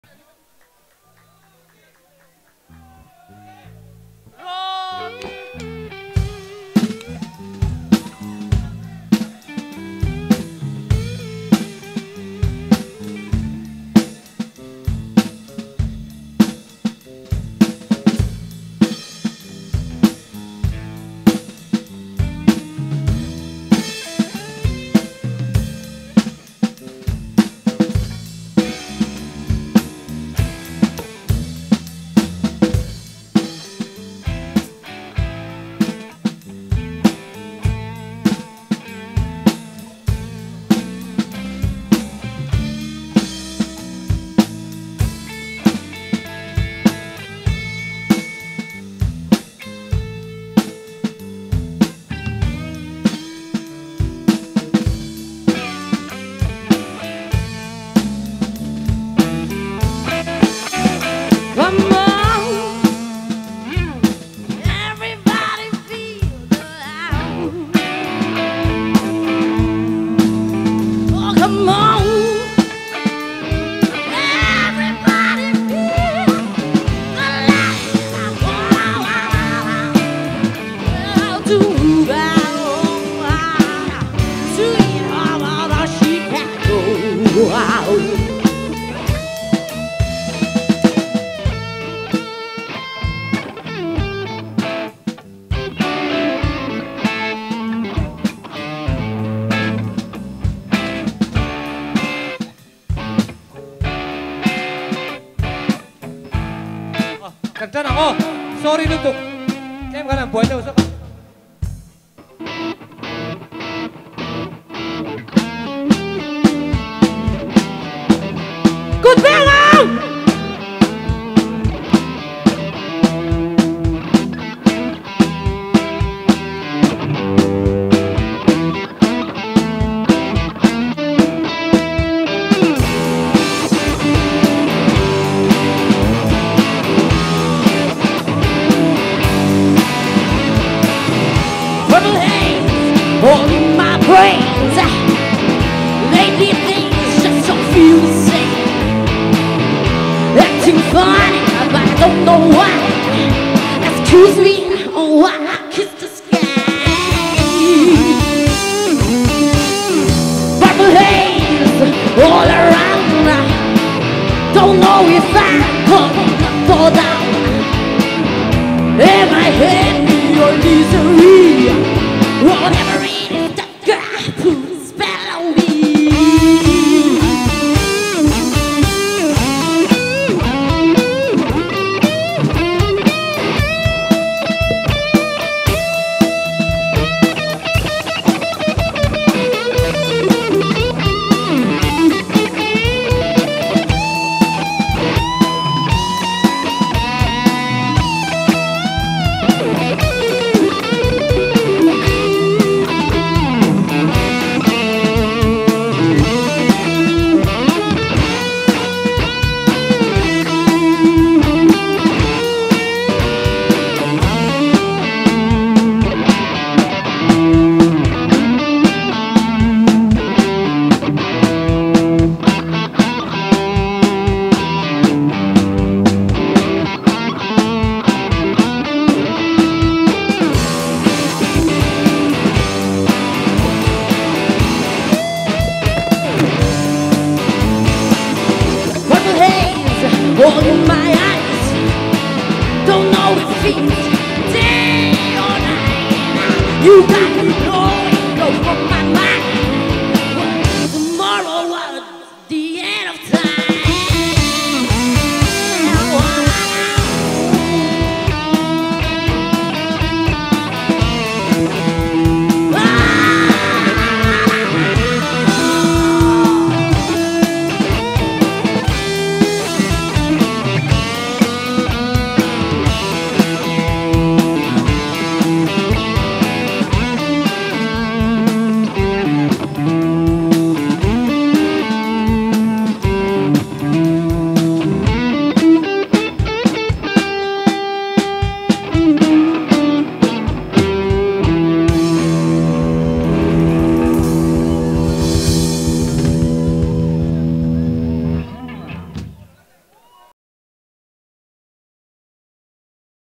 Powerful voice of a Mizo girl at the Winter Carnival Festival, Mizoram
Mizo girl performs english songs at the Winter Carnival Festival in Mizoram, Aizawl